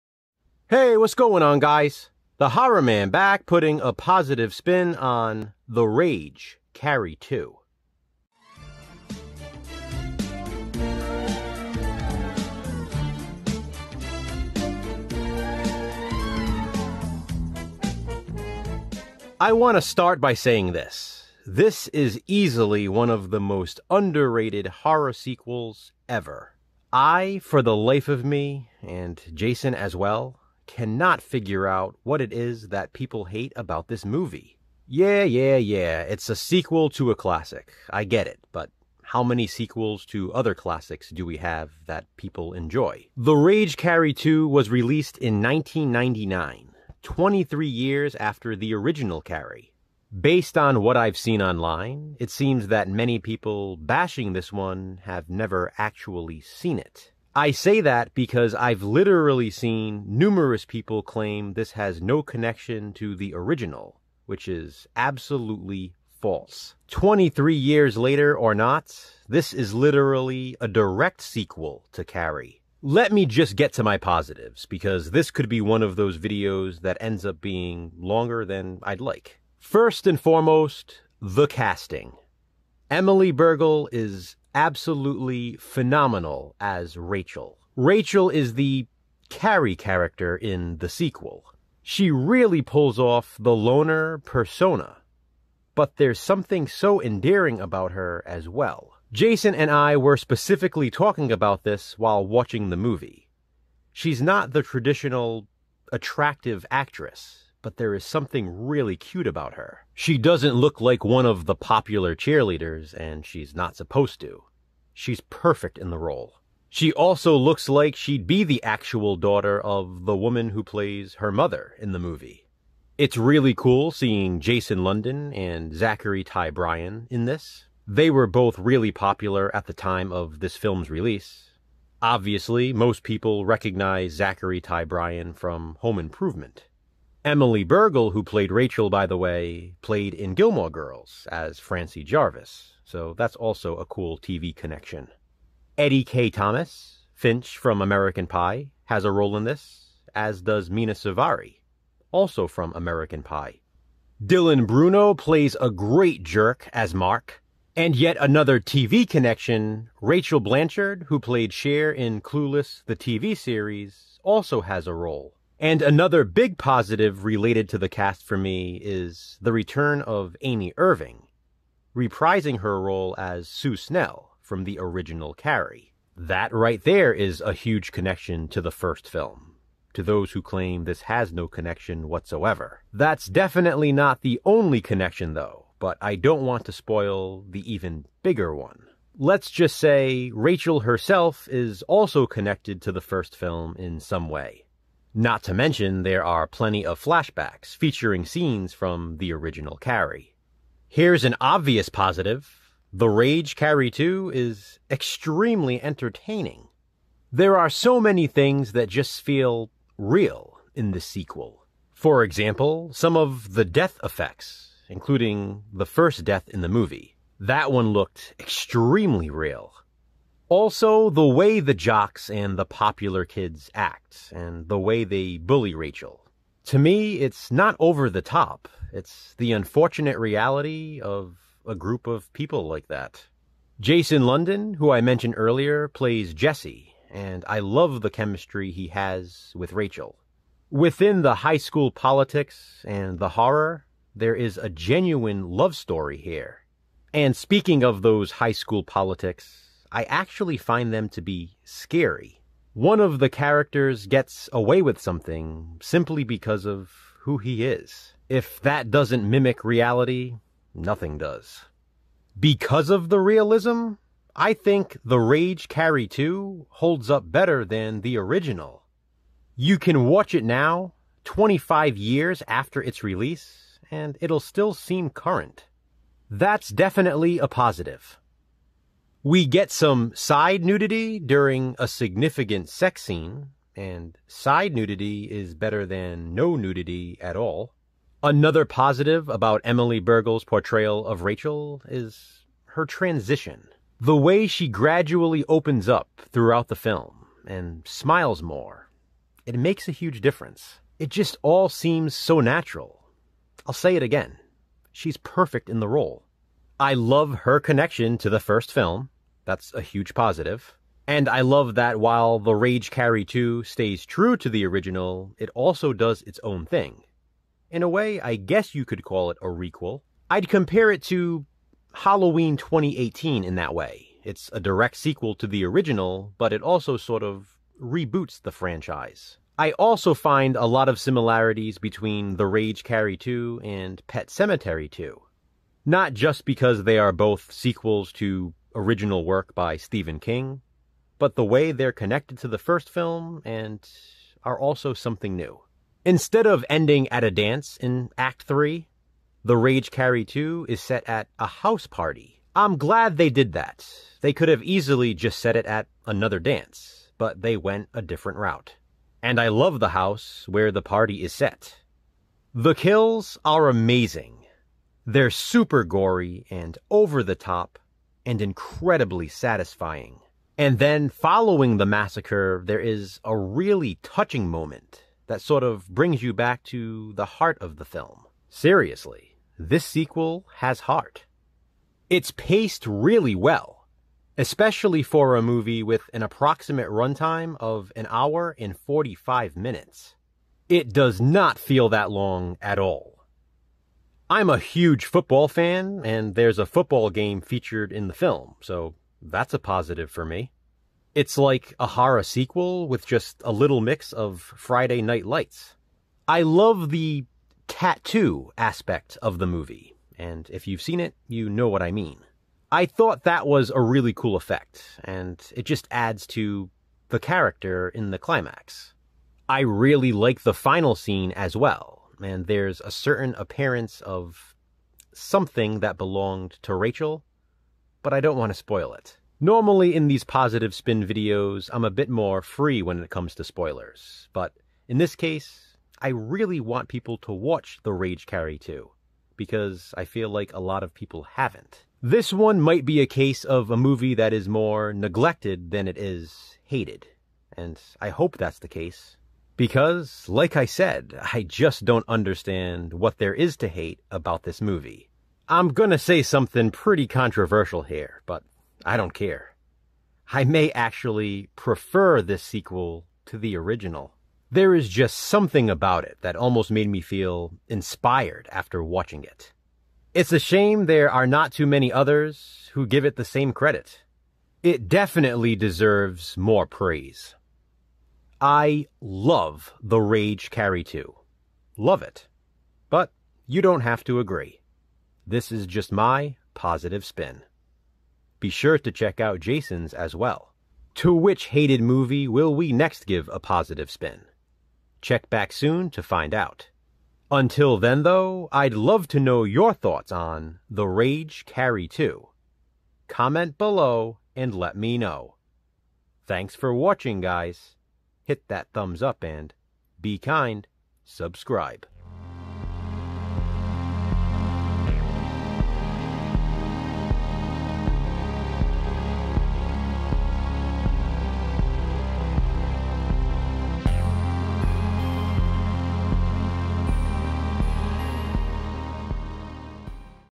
Positive Spin: THE RAGE: CARRIE 2 (1999)
Discussing the positives of THE RAGE: CARRIE 2 (1999).